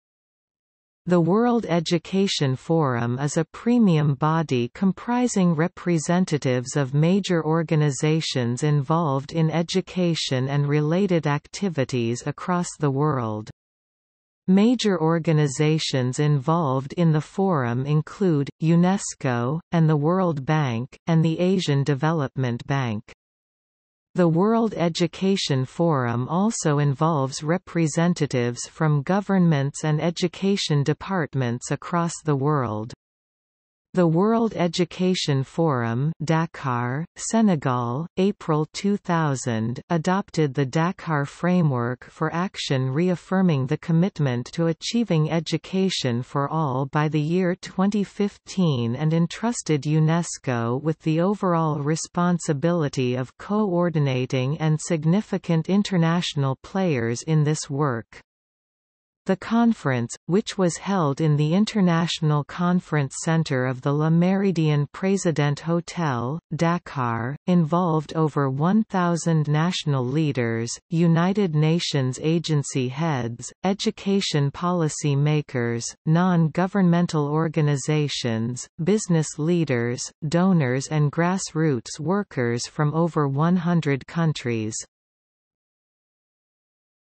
World Education Forum | Wikipedia audio article
- Socrates

SUMMARY
The World Education Forum is a premium body comprising representatives of major organisations involved in education and related activities across the world. Major organisations involved in the forum include: UNESCO, and the World Bank, and the Asian Development Bank.  The World Education Forum also involves representatives from governments and education departments across the world. The World Education Forum (Dakar, Senegal, April 2000) adopted the Dakar Framework for Action reaffirming the commitment to achieving Education for All by the year 2015 and entrusted UNESCO with the overall responsibility of co-ordinating  and significant international players in this work. The conference, which was held in the international conference centre of the Le Méridien Président Hotel, Dakar, involved over 1,000 national leaders, United Nations agency heads, education policy-makers, non- governmental organizations, business leaders, donors and grassroots workers from over 100 countries.